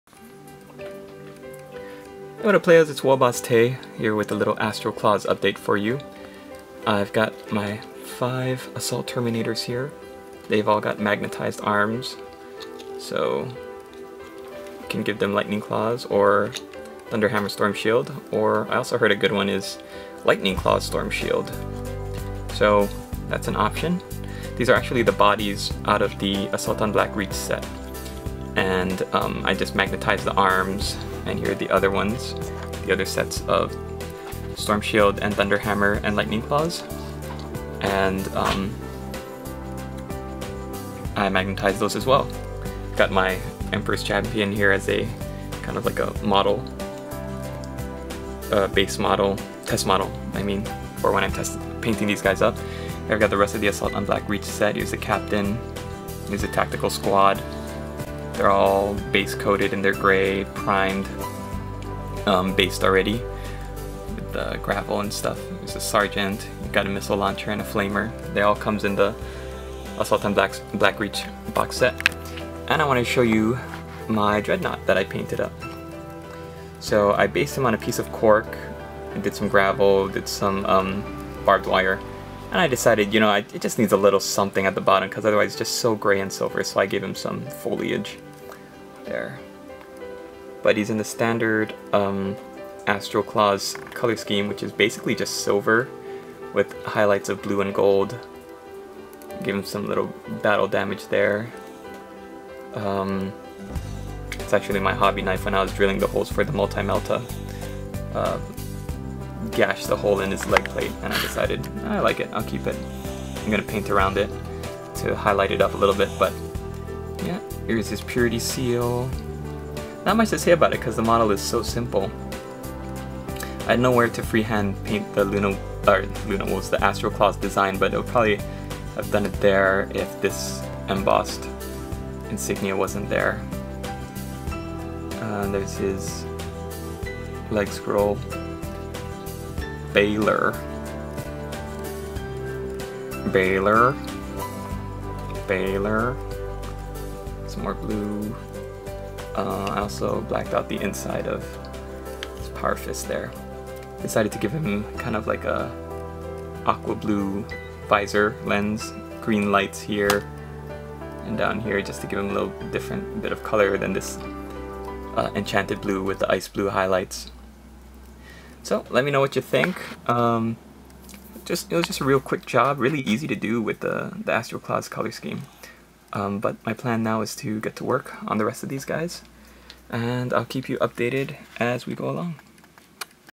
Project Astral Claws - Day 1
Today on the table, got a (just-about) fully painted Attack on Black Reach dreadnaought.  Also, I magnetized my AoBR terminators and based and primed all of the AoBR marine models, ready for painting.